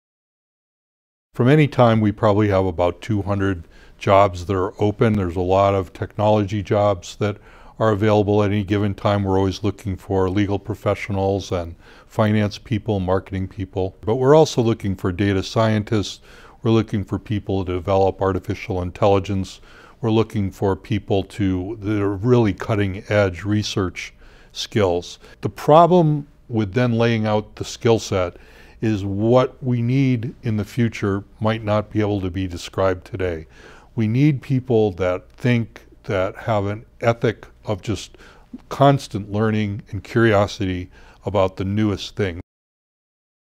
SOTC 2018: Thomson Reuters' Rick King Talks Workforce
As part of the 2018 State of the City,  we asked Thomson Reuters executive Rick King to tell us about the workforce supply challenge from the perspective of the City’s largest employer.